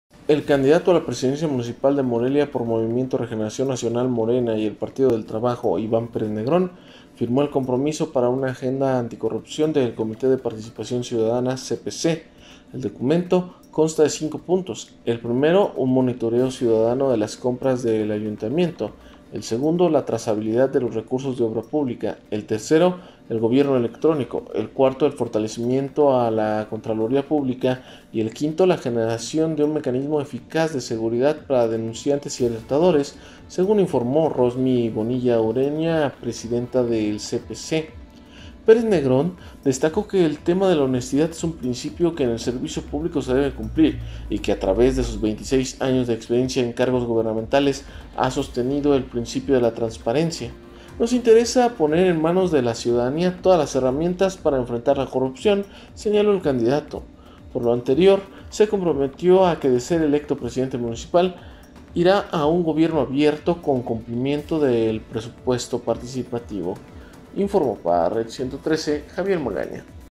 Iván Pérez Negrón se une al compromiso de una Morelia sin corrupción
Morelia, Mich., a 26 de mayo de 2021.- El candidato a la presidencia municipal de Morelia por Movimiento Regeneración Nacional (Morena) y el Partido del Trabajo (PT), Iván Pérez Negrón, firmó el Compromiso para una agenda anticorrupción del Comité de Participación Ciudadana (CPC).

El documento consta de cinco puntos, el primero un monitoreo cuidado de las compras del Ayuntamiento, el segundo la trazabilidad de los recursos de obra pública, el tercero el gobierno electrónico, el cuarto el fortalecimiento a la contraloría pública y el quinto la generación de un mecanismo eficaz de seguridad para denunciantes y alertadores, según informó Rosmi Bonilla Ureña, presidenta del CPC.

Pérez Negrón, destacó que el tema de la honestidad es un principio que en el servicio público se debe cumplir y que a través de sus 26 años de experiencia en cargos gubernamentales ha sostenido el principio de la transparencia.

"Nos interesa poner en manos de l ciudadanía todas las herramientas para enfrentar la corrupción", señaló el candidato.

Por lo anterior se comprometió a que de ser electo presidente municipal irá a un gobierno abierto con cumplimiento del presupuesto participativo.